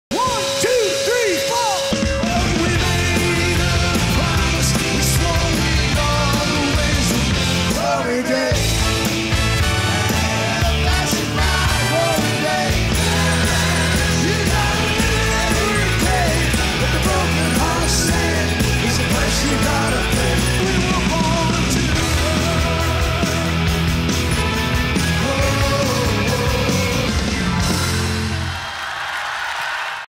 Bruce Springsteen and The E Street Band le 24 Mai 2025 à Lille au Stade Pierre Mauroy
Événement !! Bruce Springsteen and The E Street Band annonce un nouveau concert en France samedi 24 Mai 2025 à Lille au Stade Pierre Mauroy.

Mise en vente des places lundi 7 octobre dès 10h sur Gérard Drouot Productions, Fnac stade et Ticketmaster France